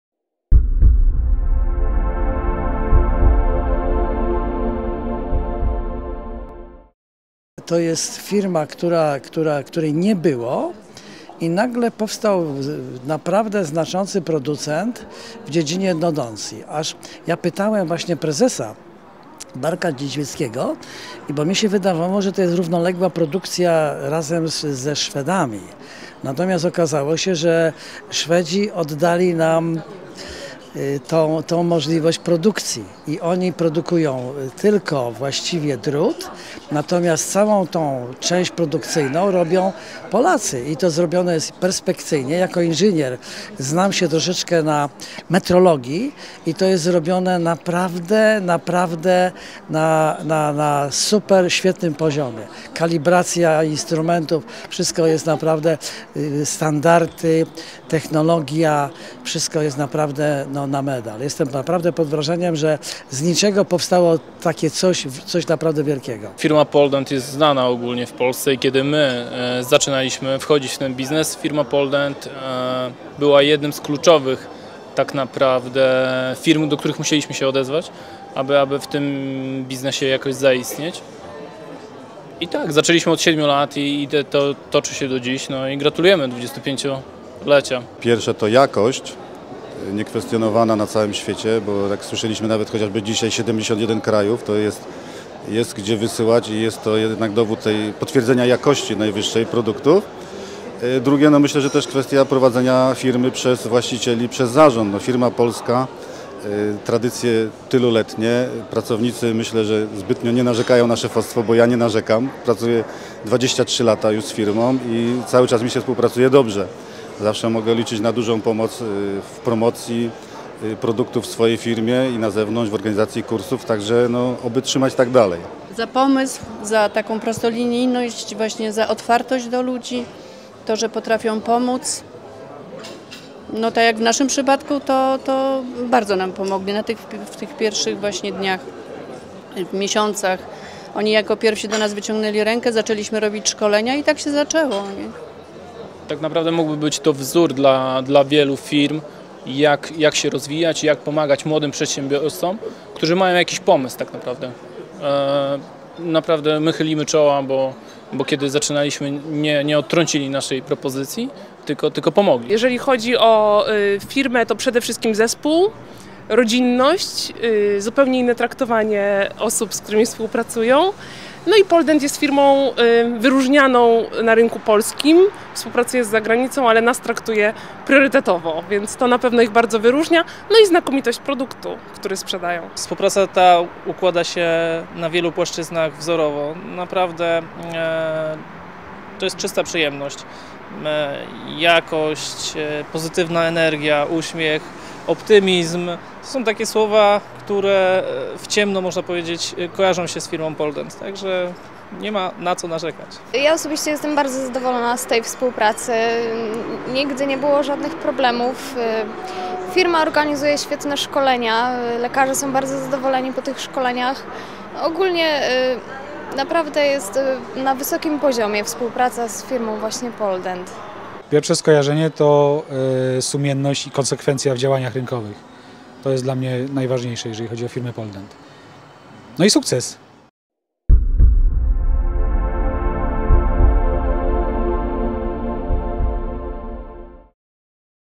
POLDENT 25 lat - Współpraca z firmami
Prezentujemy wypowiedzi firm współpracujących w Poldent. Sprawdźcie, jak się z nami współpracuje! :)